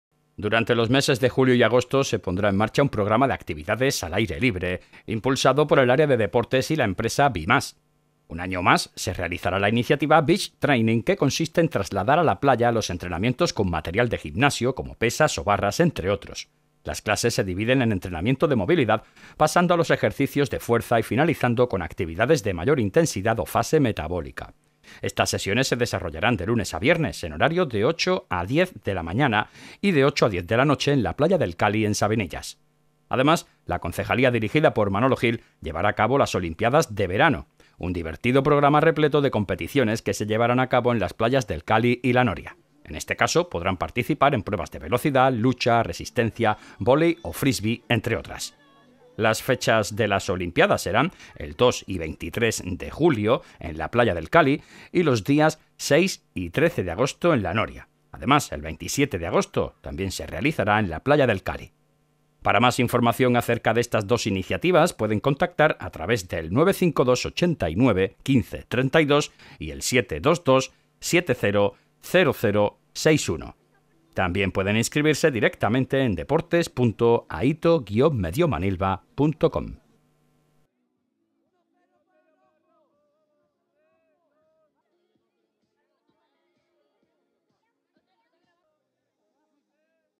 Programa de actividades al aire libre
Durante los meses de julio y agosto se pondrá en marcha un programa de actividades al aire libre, puesto en marcha por el área de Deportes y la empresa Be+.
Un año más se realizará la iniciativa Beach Training, que consiste en trasladar a la playa los entrenamientos con material de gimnasio, como pesas o barras, entre otros. Las clases se dividen en entrenamiento de movilidad, pasando a los ejercicios de fuerza y finalizando con actividades de mayor intensidad o fase metabólica.
Estas sesiones se desarrollarán de lunes a viernes en horario de 8:00 a 10:00 h. y de 20:00 a 22:00 h. en la playa del Cali en Sabinillas.
Además, la concejalía dirigida por Manolo Gil, llevará a cabo las Olimpiadas Verano. Un divertido programa repleto de competiciones que tendrán lugar en las playas del Cali y La Noria.

En este caso podrán participar en pruebas de velocidad, lucha, resistencia, voley o frisbie, entre otras.
Las fechas de las Olimpiadas serán el 2 y 23 de julio (19:30 a 21:30 h.) en la playa del Cali, los días 6 y 13 de agosto en La Noria, y el 27 de agosto en el Cali (19:00 a 21:00 h.)
Para más información acerca de estas dos iniciativas pueden contactar a través del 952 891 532 y el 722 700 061. También pueden inscribirse directamente en Deportes.ayto-manilva.com.